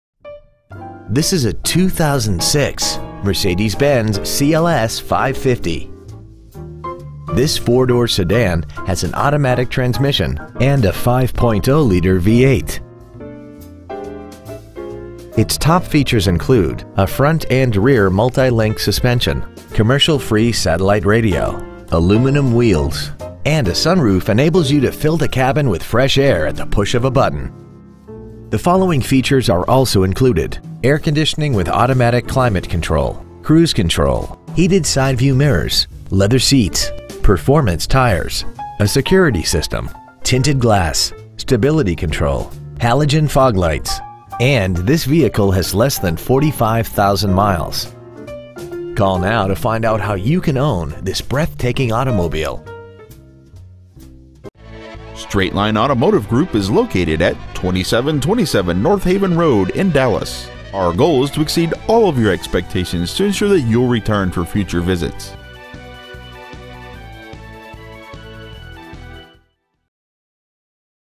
2006 Mercedes-Benz CLS550 Dallas TX
We are proud to present this 2006 Mercedes-Benz CLS550 .

 Year : 2006
 Make : Mercedes-Benz
 Model : CLS550
 Engine : 5.0L SOHC SMPI 24-valve V8 engine
 Trans . : Automatic
 Exterior : Gray
 Miles : 44,879
 Interior : Gray